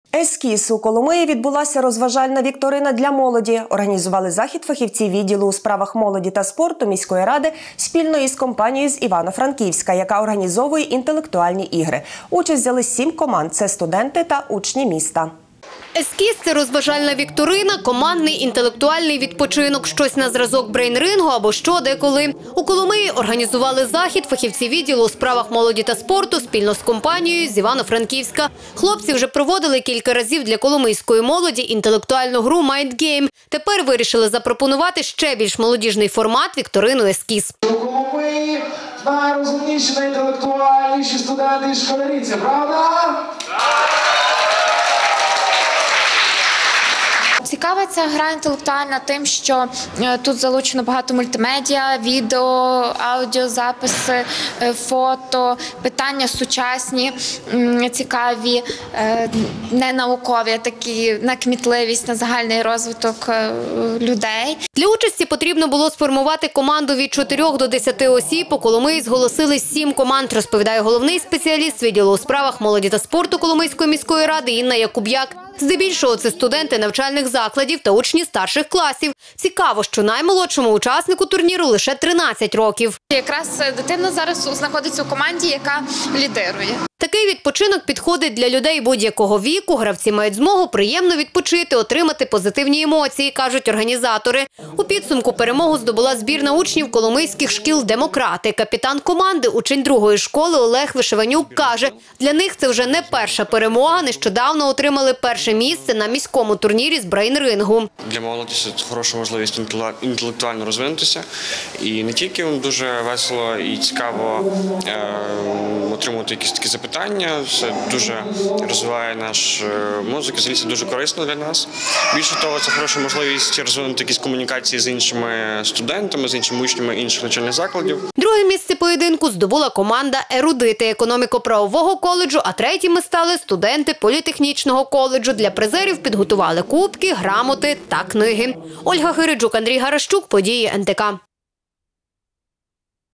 У Коломиї відбулася розважальна вікторина для молоді
Організували захід фахівці відділу у справах молоді та спорту міської ради, спільно із компанією з Івано-Франківська, яка організовує інтелектуальні ігри.

Участь взяли 7 команд. Це студенти та учні міста.